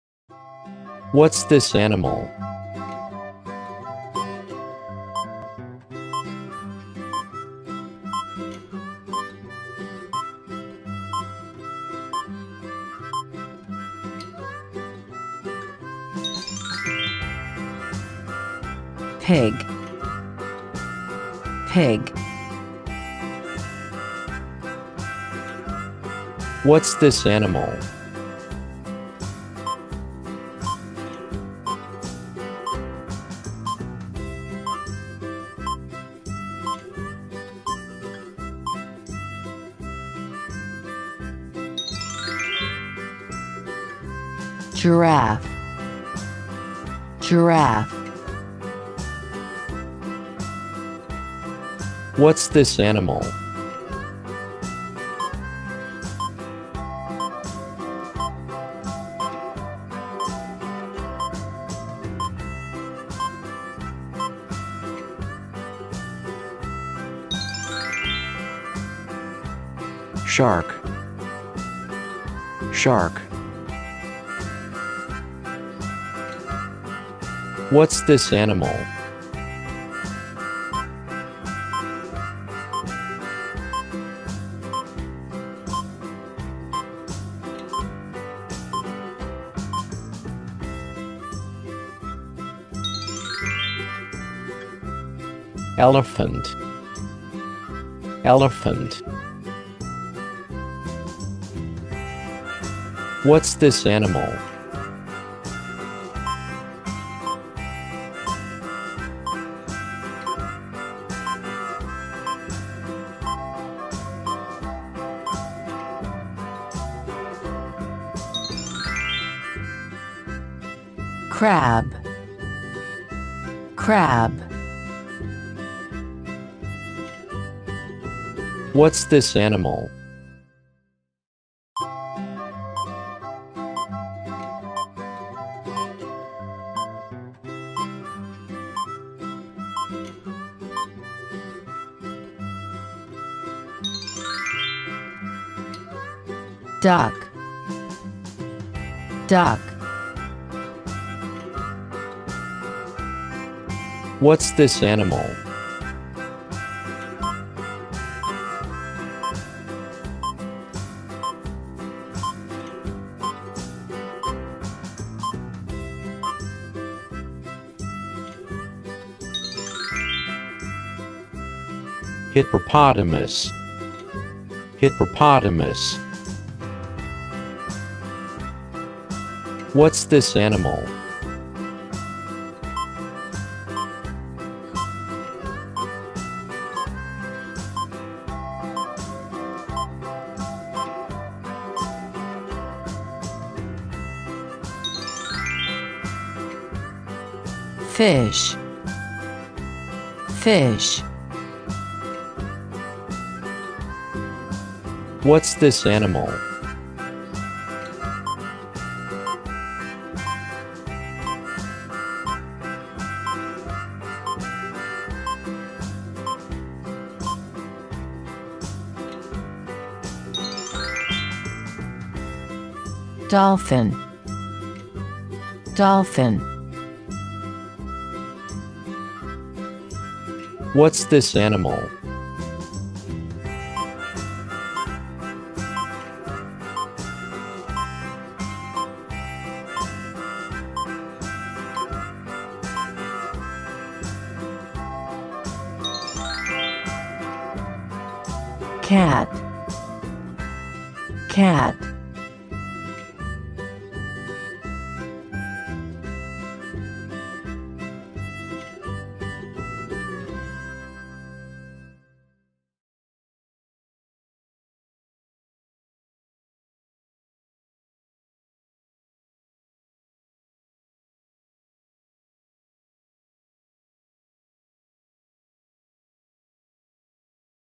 What's this Animal? Learn Animal Names with Animal Toys!
Educational video for kids, babies, toddlers, and children to learn Animals with Animal Quiz!